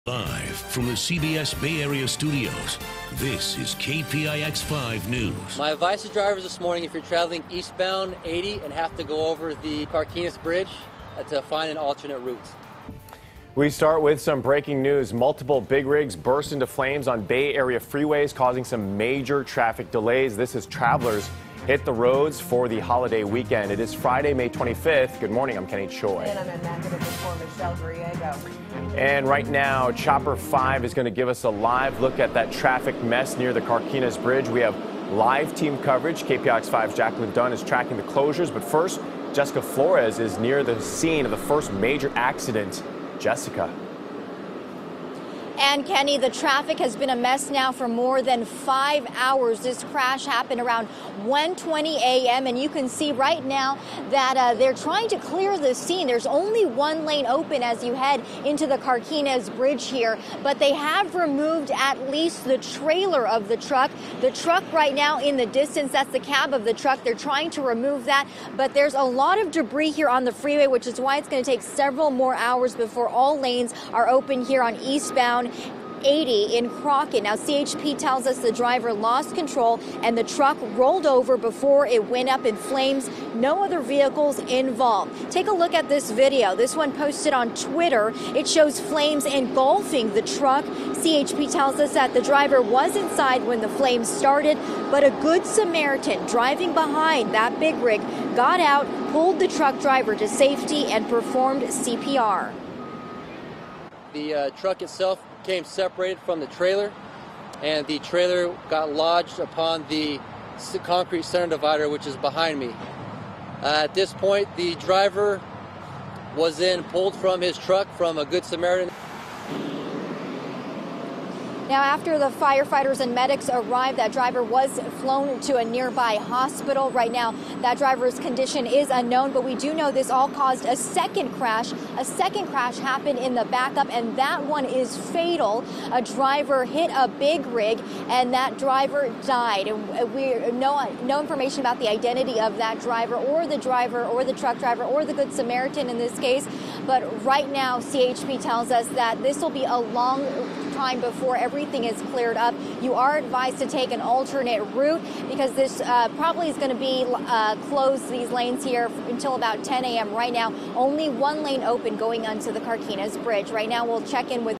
FIERY CRASH: A big rig overturned and burst into flames on I-80 near Crockett Friday morning
A big rig overturned and burst into flames on I-80 near Crockett Friday morning